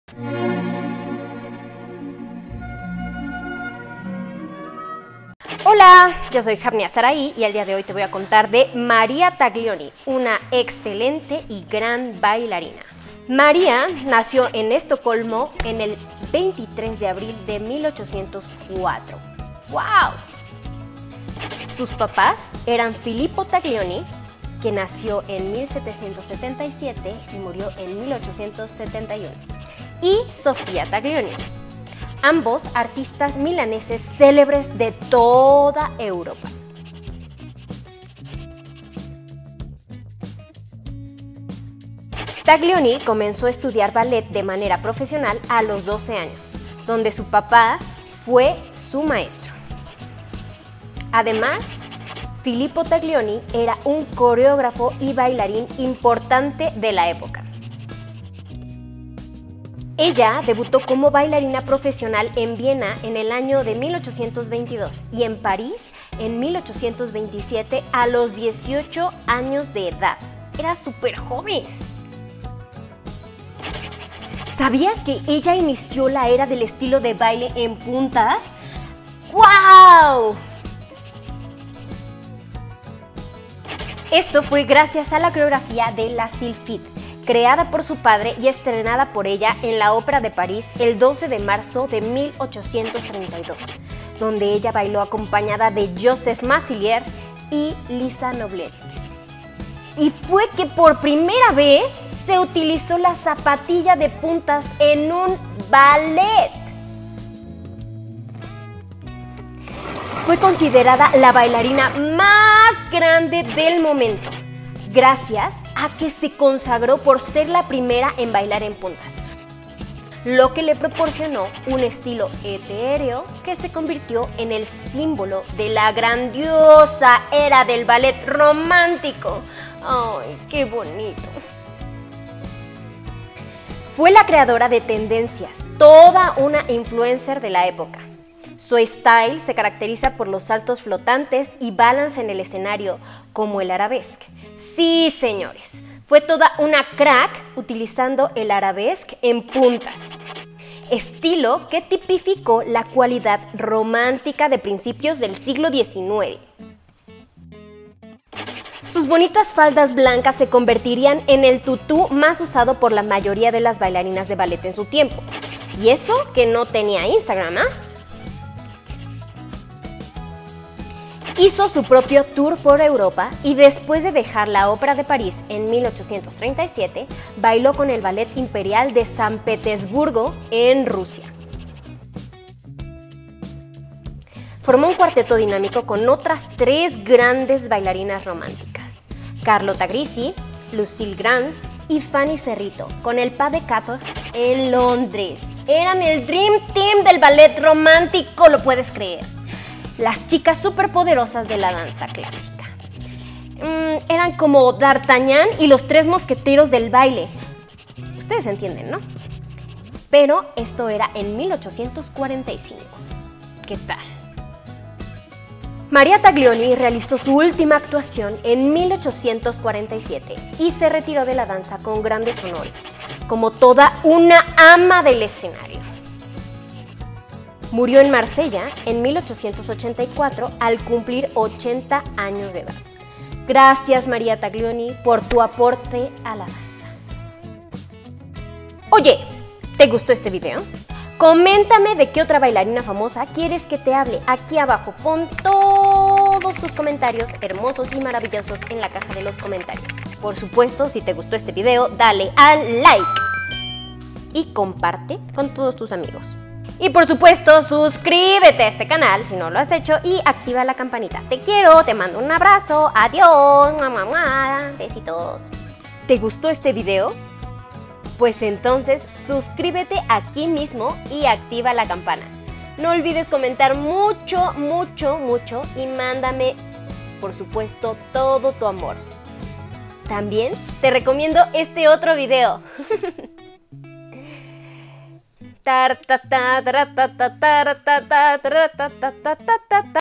🔥 MARIA TAGLIONI (La primera BAILARINA en Subirse en PUNTAS) | Madame Marie Taglioni (en 4 min)🔥
💖 Querida Familia de Ballet Tips, hoy les compartimos la bella historia de una gran bailarina ¡Maria Taglioni!
¡Esto lo debes saber si eres amante de la Danza y el Ballet!